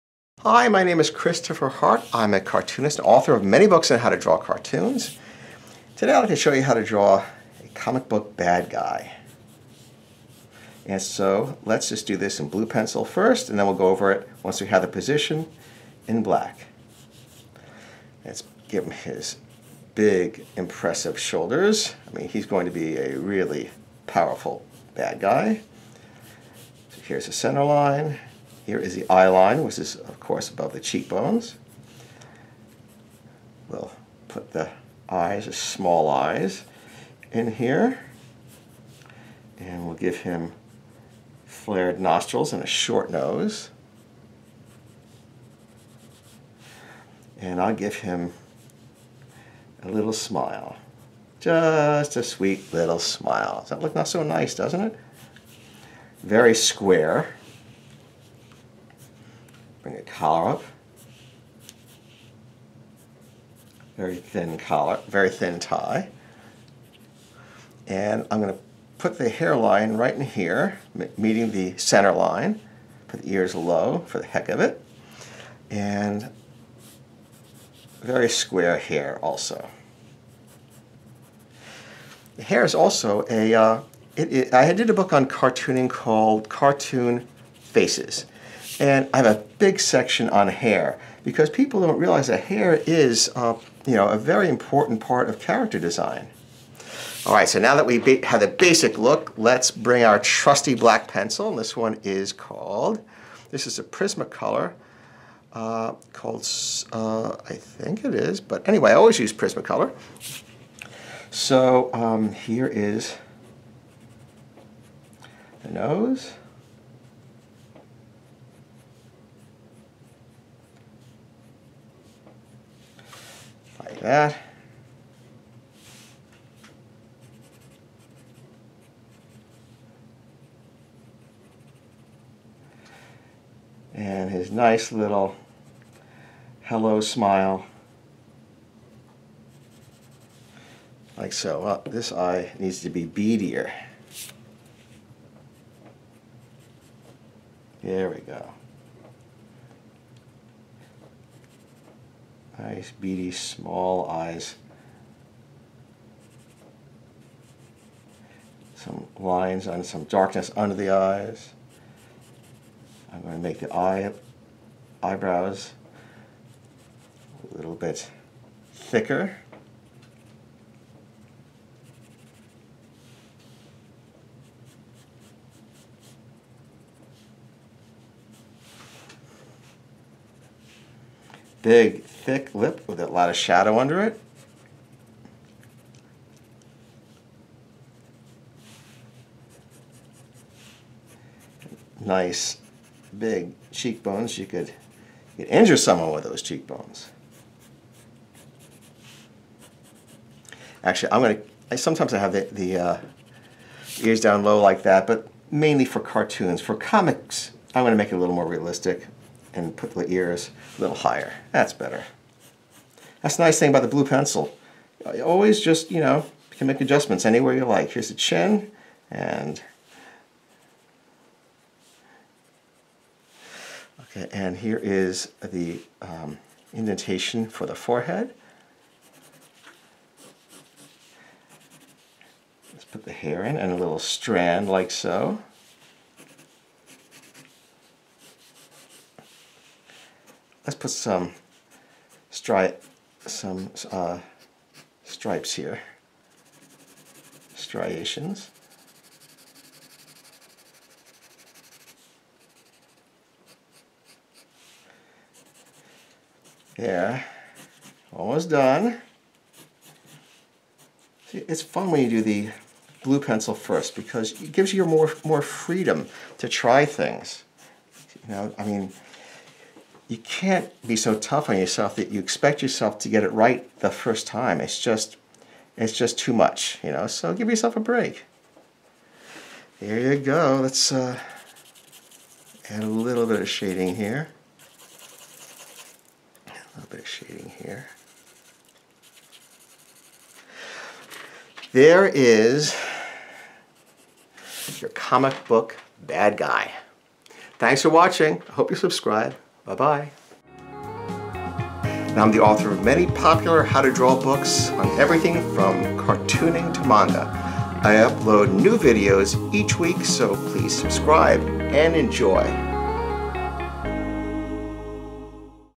Draw a Bad Guy
Caution: children should use child-safe art supplies. Hi friends & loyal readers of my how-to-draw books. Here's a fun tutorial for you: how to draw a bad guy. Villains and other miscreants are fun to draw because we can exaggerate their head shapes and features. Notice how small the top half of the head is (cranium), and ow short the nose is. Why do I draw it that way? In order to exaggerate the size of the chin, to make him look powerful. Also, he has no neck, which brings up the shoulders, increasing the stocky look - another powerful aspect of the characters. Give it a try. I think you'll enjoy drawing this cartoons. Have fun!